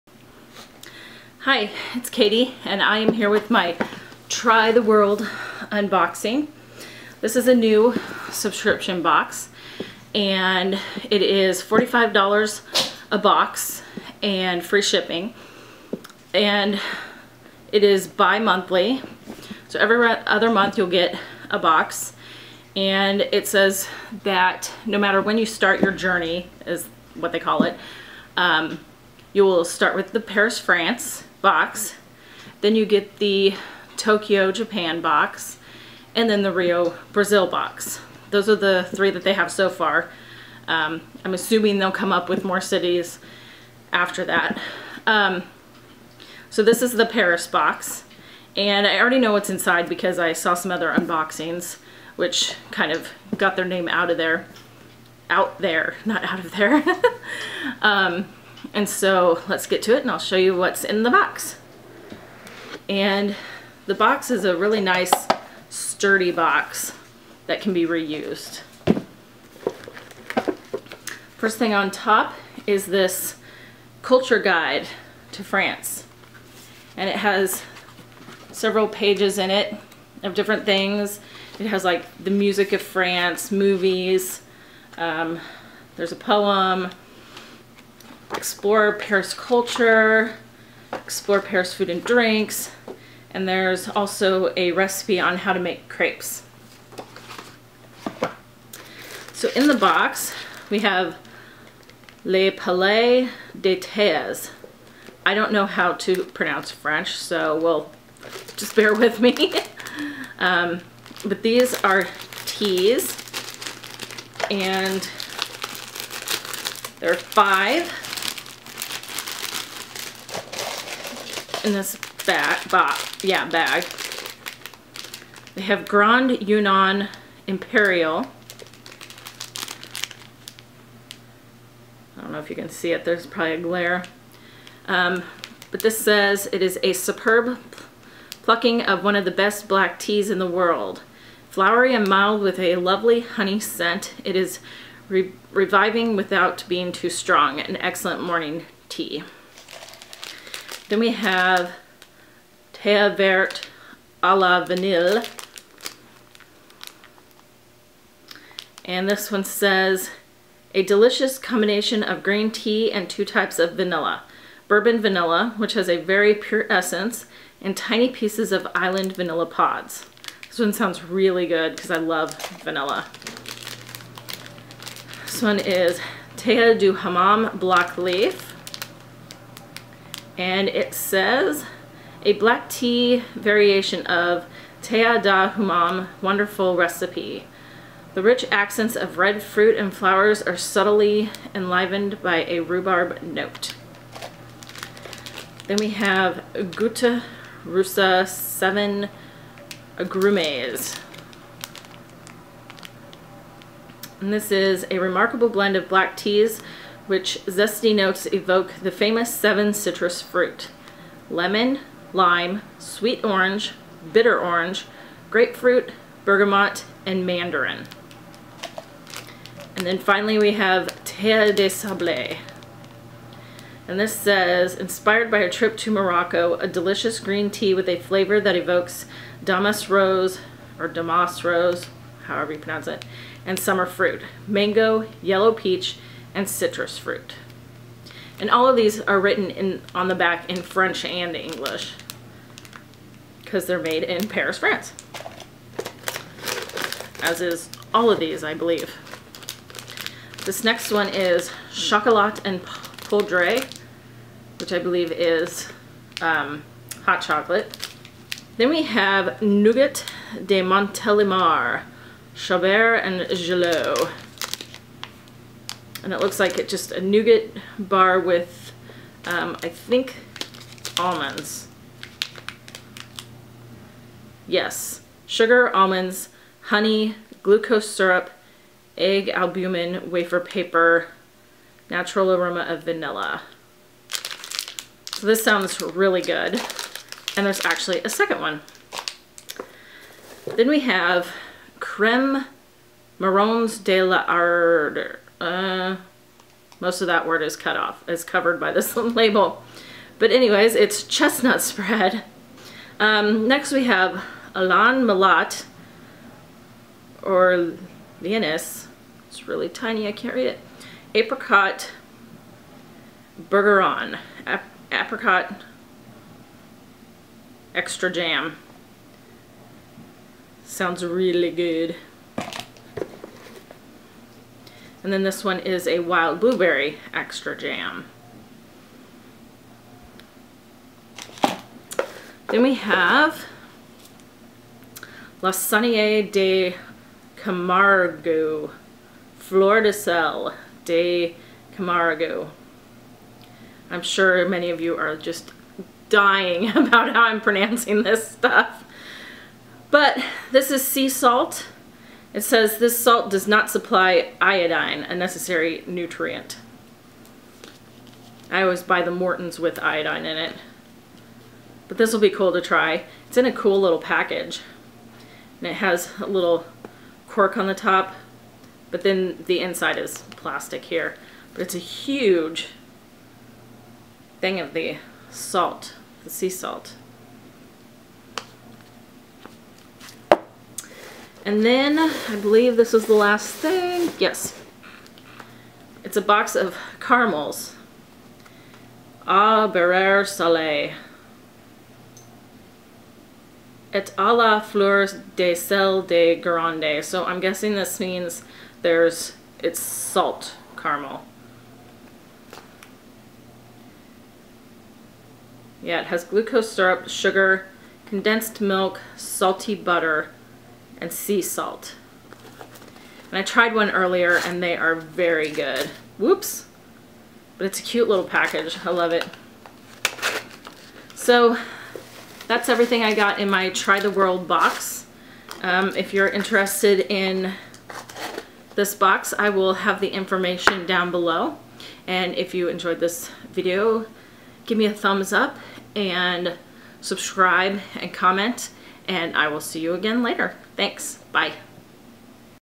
Try The World-Paris Unboxing 2014
Try The World is a bimonthly box. It is $45 including shipping.